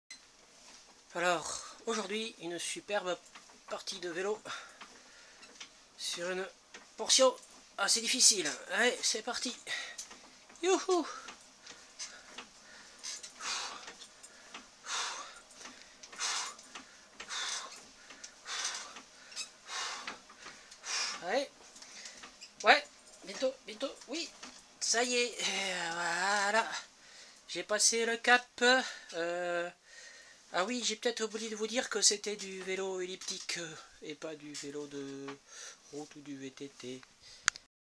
J'ai filmé ma dernière sortie à vélo
Un petit film sur un superbe passage difficile à vélo, le niveau 8.